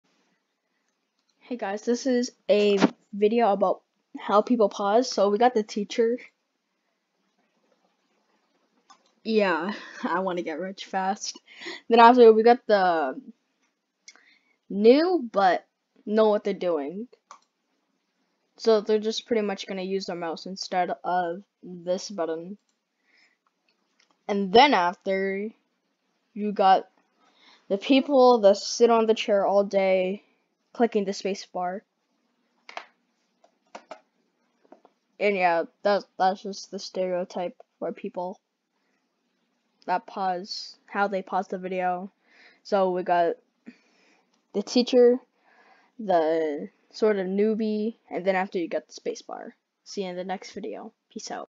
How People Pause A Video Stereotypes
Its true I watch my teacher goes to the pause button and pause the vid like who does that oh wait I was just talking it dang it
it seems true how this stereotype works